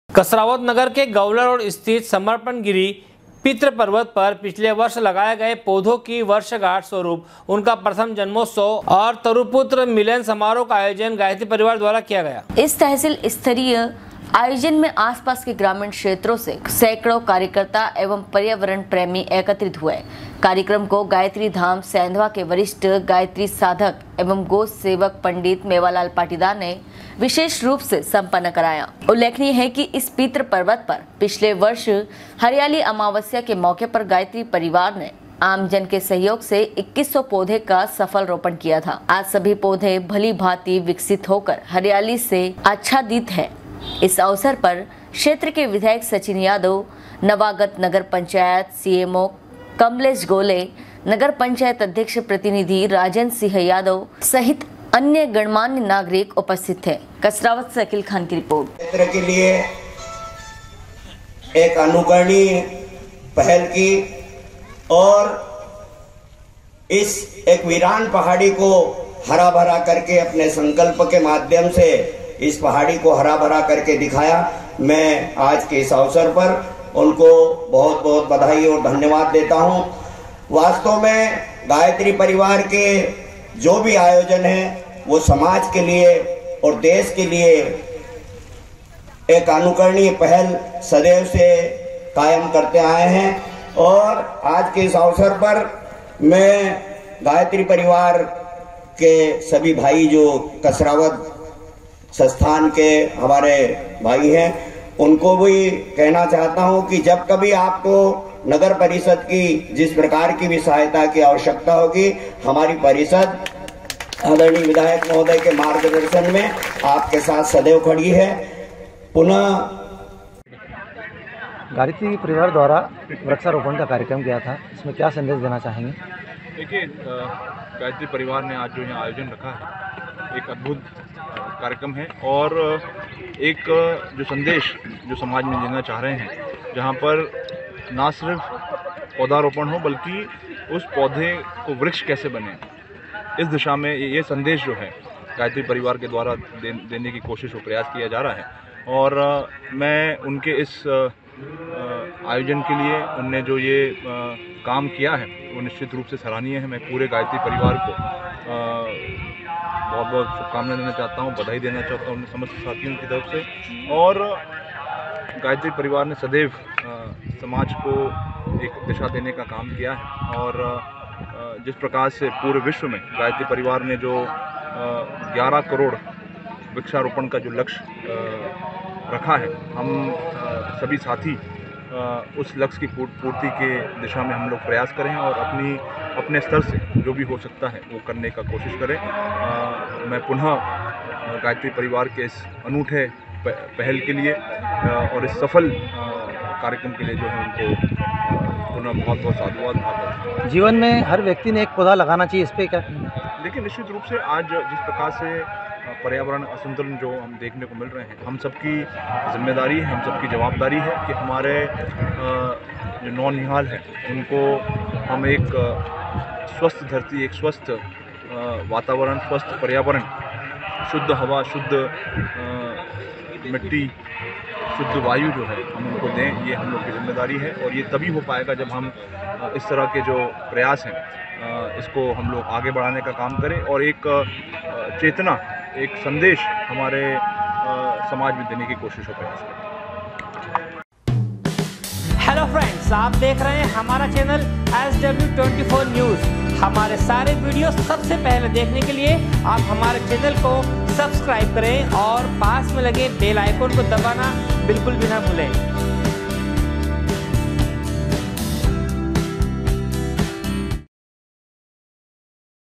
नगर के गवला रोड़ स्थित समर्पण गिरि (पितृ पर्वत) पर पिछले वर्ष लगाए गए पौधों की वर्षगांठ स्वरूप
नगर के गवला रोड़ स्थित समर्पण गिरि (पितृ पर्वत) पर पिछले वर्ष लगाए गए पौधों की वर्षगांठ स्वरूप उनका प्रथम जन्मोत्सव एवं तरूपुत्र मिलन समारोह का आयोजन गायत्री परिवार द्वारा किया गया।
कसरावद से अकिल खान की रिर्पोट।